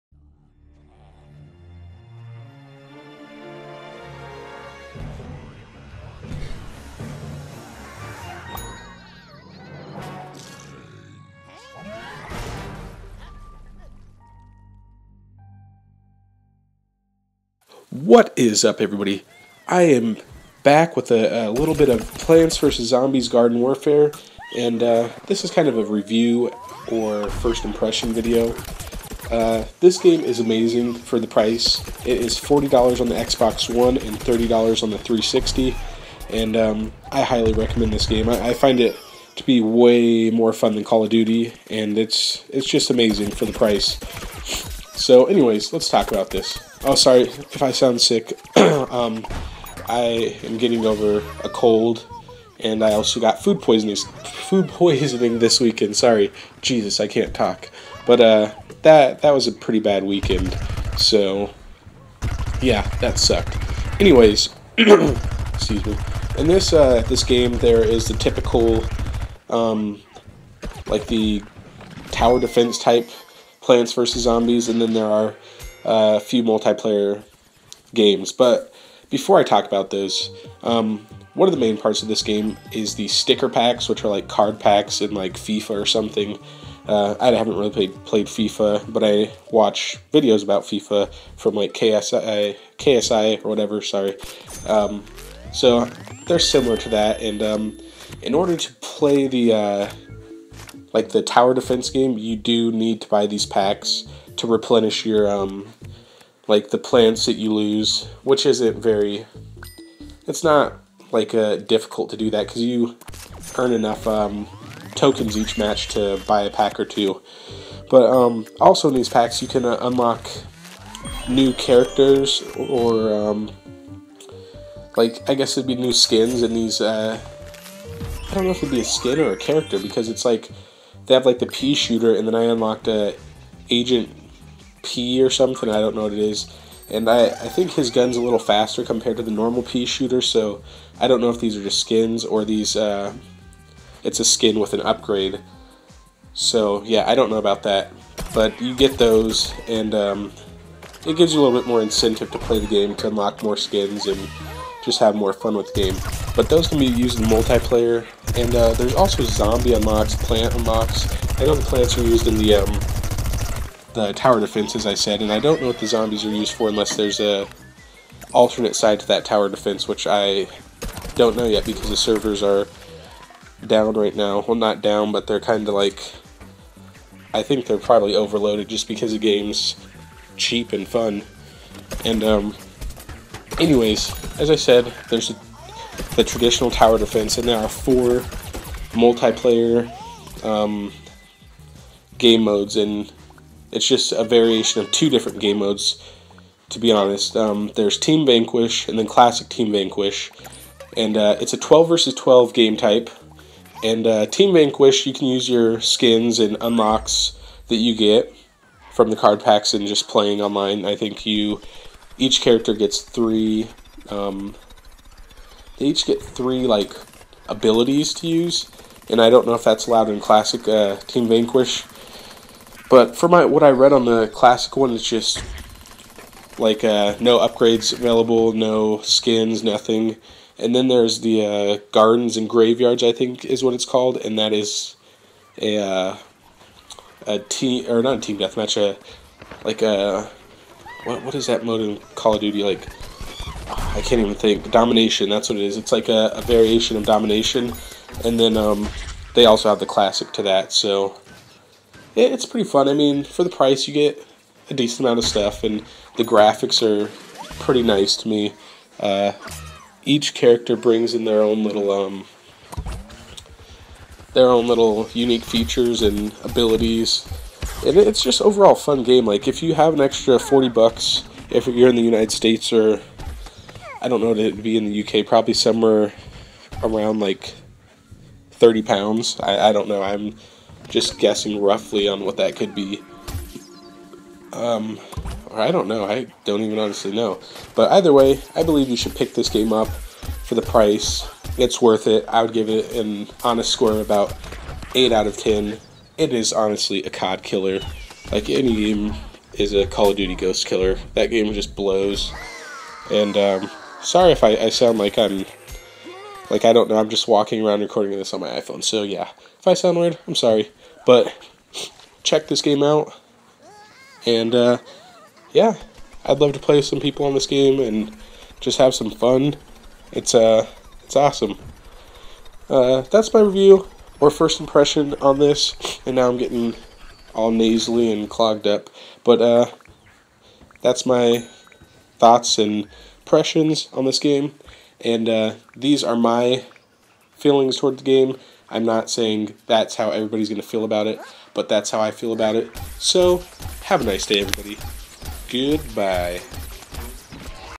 Plants Vs Zombies Garden Warfare Review / Thoughts
I LOVE this game and would recommend it to everyone.

Hope you enjoy this game as much as i do :)